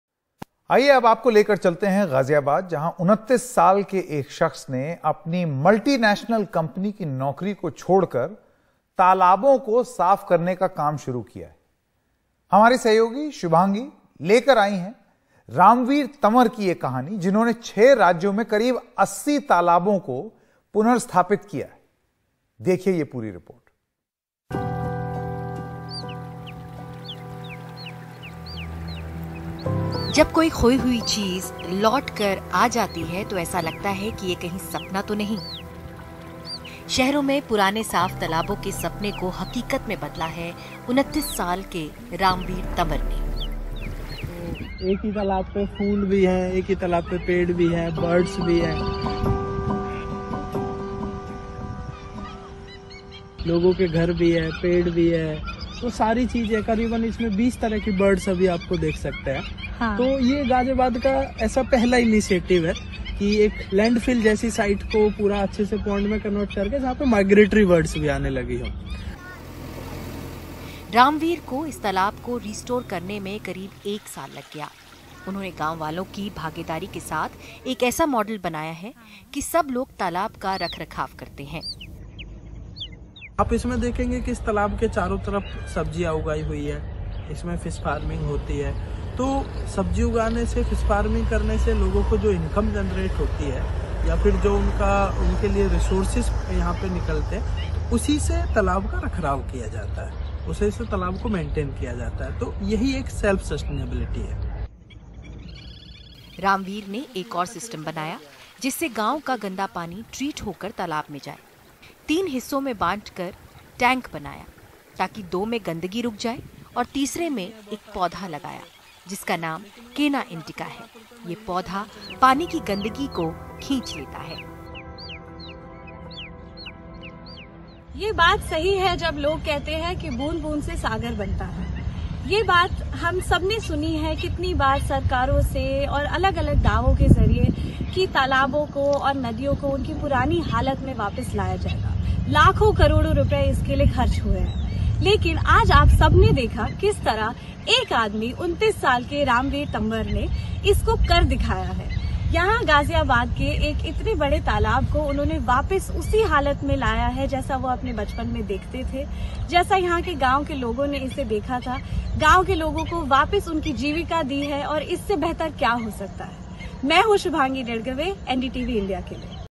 मिलिए Pond Man से, MNC की नौकरी छोड़कर शुरू किया तालाबों को साफ करने का काम
गाजियाबाद निवासी 29 वर्षीय रामवीर तंवर ने अपनी मल्टीनेशनल कंपनी की नौकरी को छोड़कर तालाबों को साफ करने का काम शुरू किया. उन्होंने छह राज्यों में करीब अस्सी तालाबों को पुनर्स्थापित किया. देखिए ये पूरी रिपोर्ट.